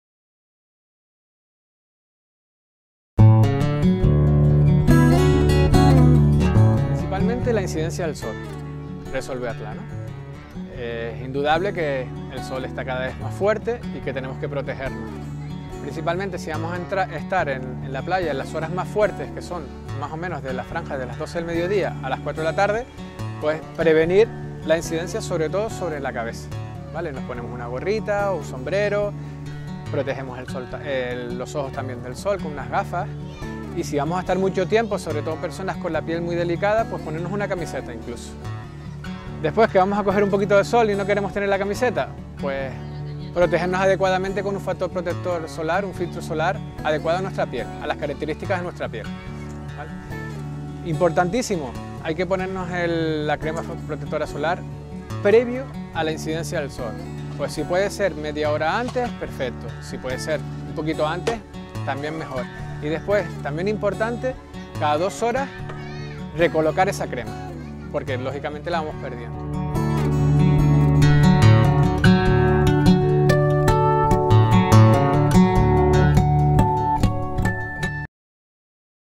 Las lesiones por el sol son muy habituales en la playa
En la playa debemos protegernos del sol: lociones solares, gorros y gafas para evitar lesiones. Sigue los consejos de Óliver Castro, Coordinador de Salvamento Marítimo de Cruz Roja